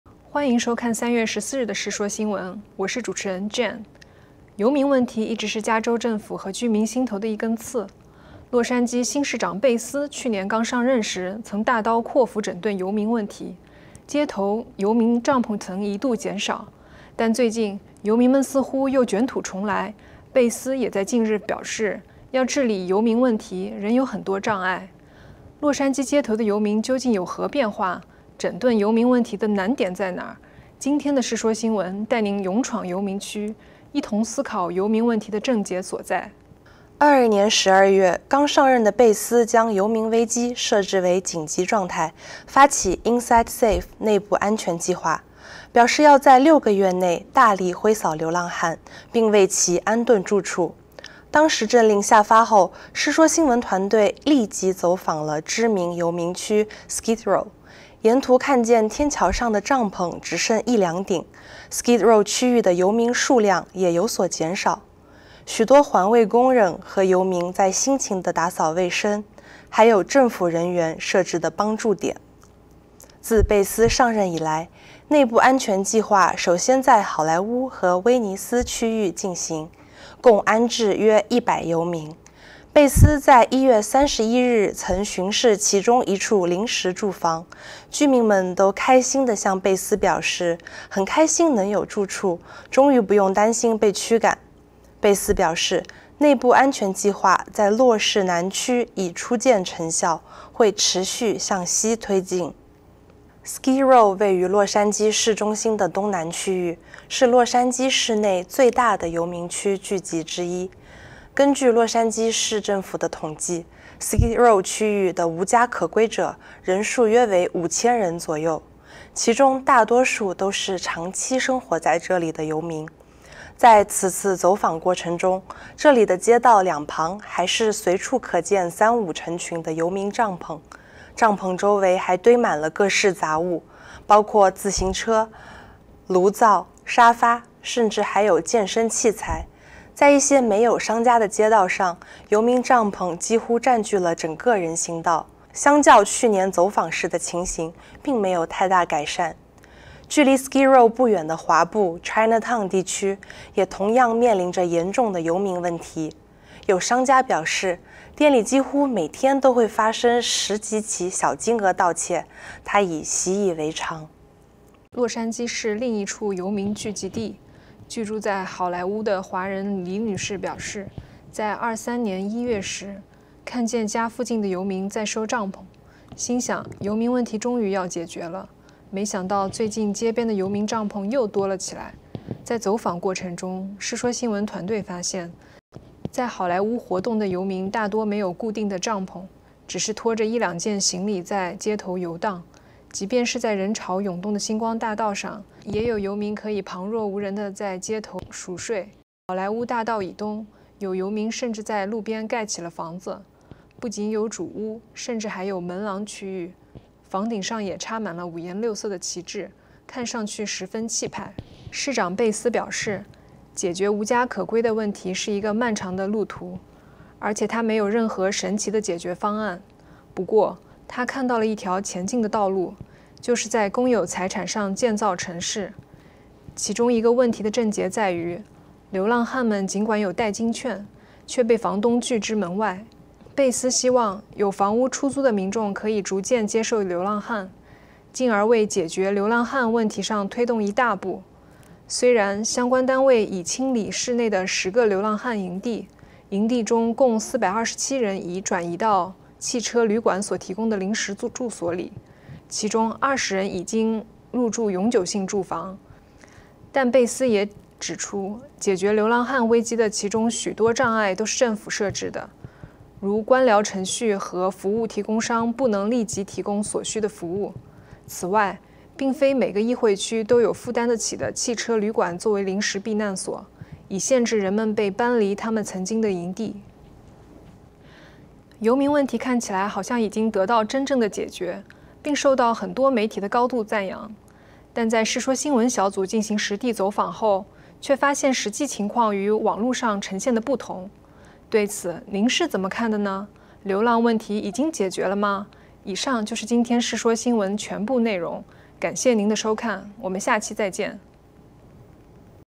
世說新聞／再闖最恐怖遊民區 不減反增 還在街邊幹這...
遊民問題一直是加州政府和居民心頭的一根刺，洛杉磯新市長貝斯去年剛上任時曾大刀闊斧整頓遊民問題，街頭的遊民帳篷曾一度變少，但最近遊民們似乎又捲土重來，貝斯也在近日表示，要治理遊民問題，仍有很多障礙。洛杉磯街頭的遊民究竟有何變化？整頓遊民問題的難點在哪？

0:00 今日看點
0:36 Inside Safe計劃進展
1:43 勇闖遊民區Skid Row
2:44 勇闖遊民區Hollywood
3:31 貝斯政府對策
4:37 總結